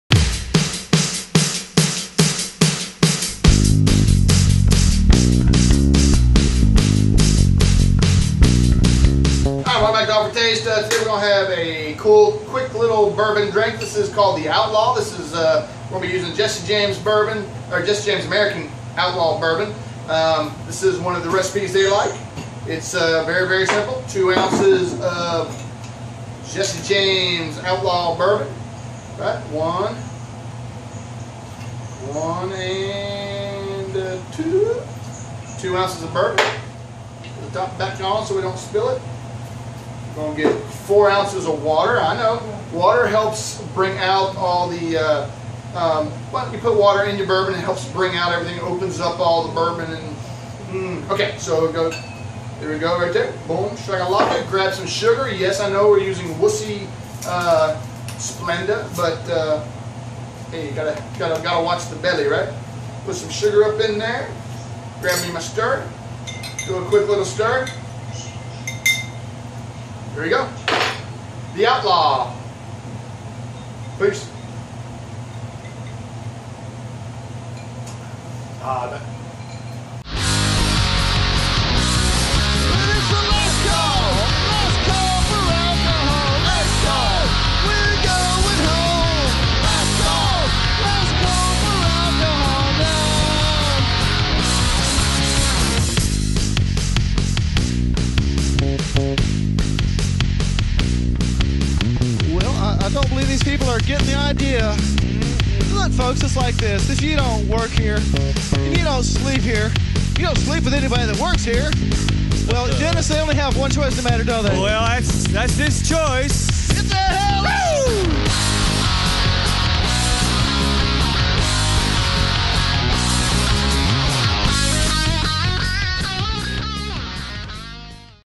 The Outlaw featuring Jesse James American Outlaw Bourbon
The Outlaw
Many enthusiasts swear by a splash of water to open up a goood Bourbon and allow for amplified enjoyment of the smokey flavor profile. Of course old Outlaws likely didn't have easy access to cola or even ice for that matter!

Add a 2 oz. Slug of JJ Bourbon to 4 oz. of water. If desired add sugar to taste. Serve straight up or on the rocks -- drinker's preference.